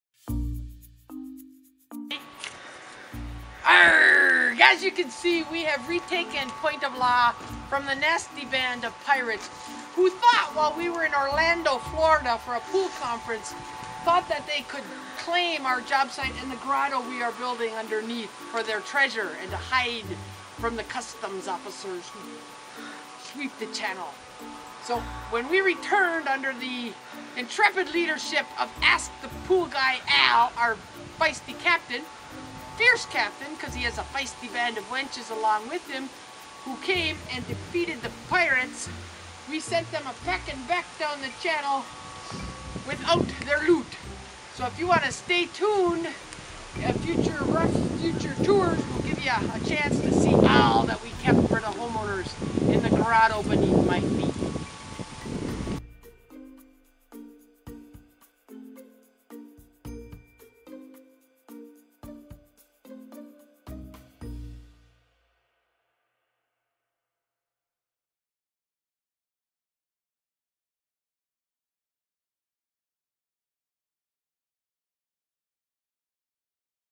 Ask the Pool Guy's Pirate Invasion with Karen {Legendary Escapes}
During our recent trip to the Int'l Pool Spa and Patio Expo in Orlando, our site was taken over by Pirates...here's developing news in the story from our trusty First Mate Karen.